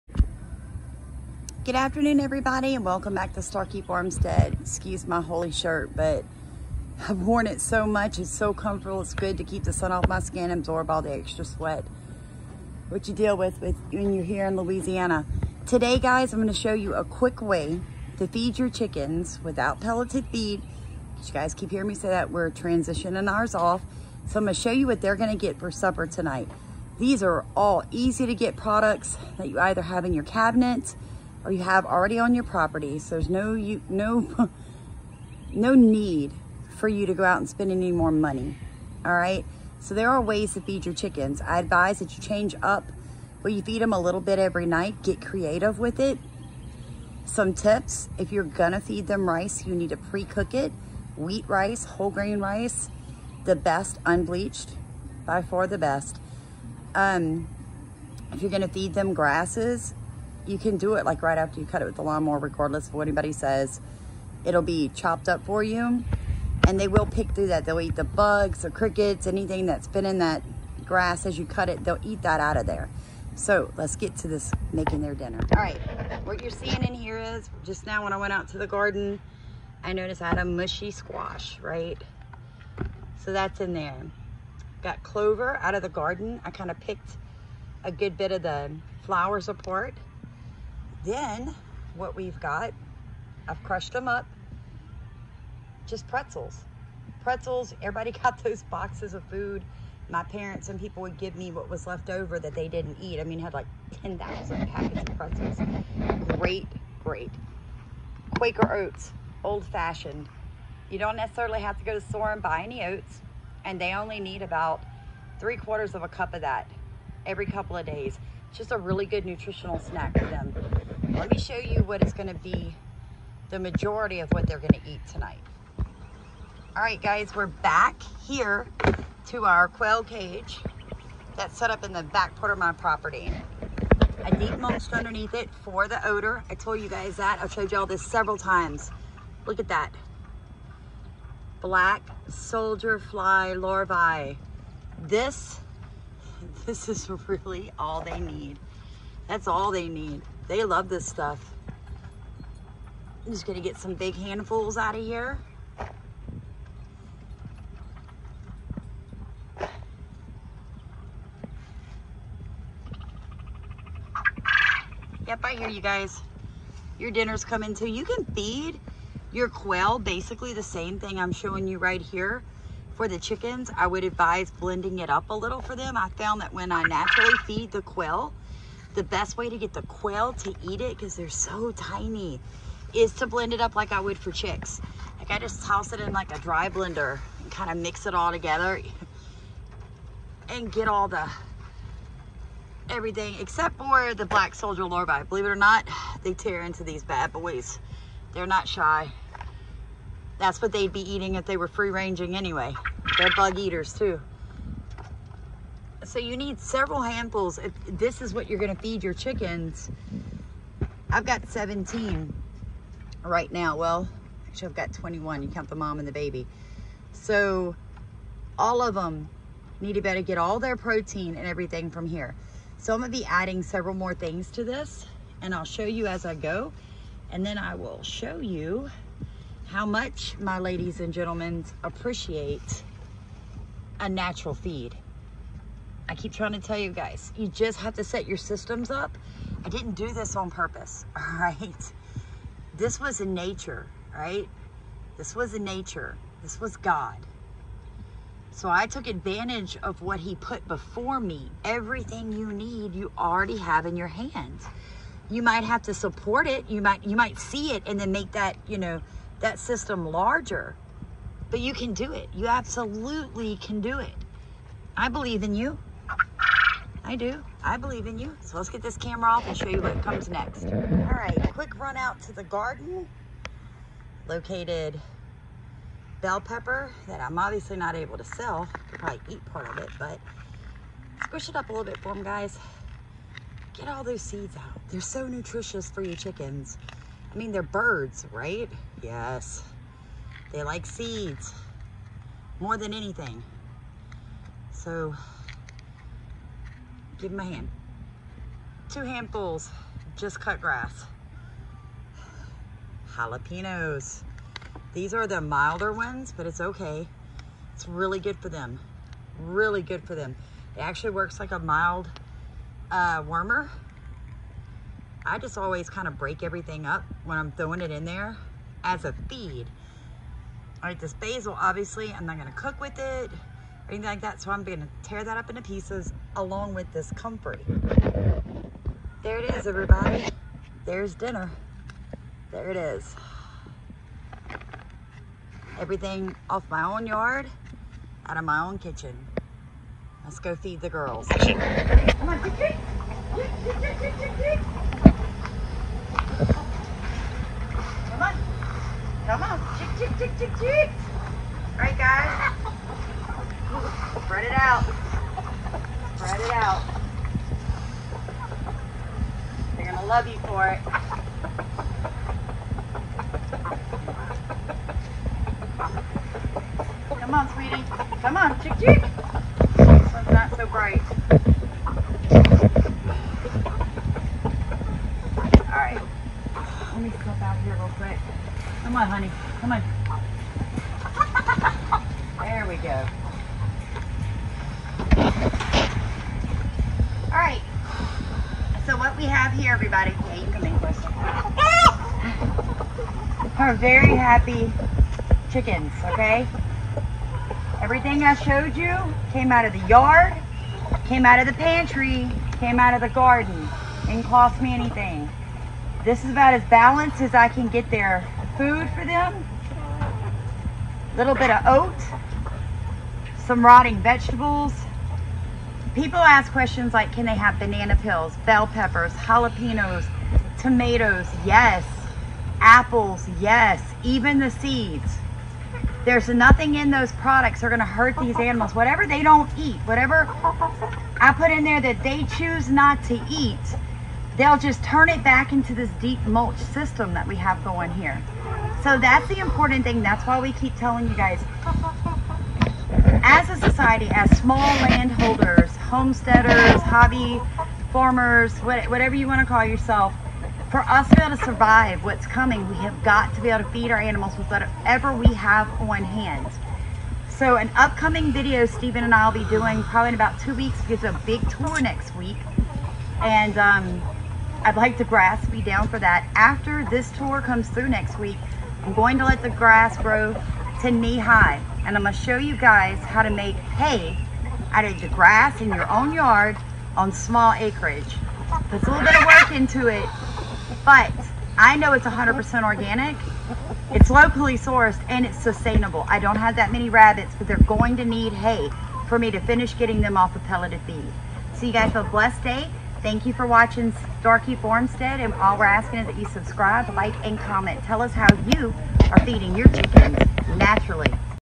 Homemade Chicken & Quail Feed Using What You Already Have In Your Home and Off Your Property!
We show you how to feed your chickens and quail w/o spending any money!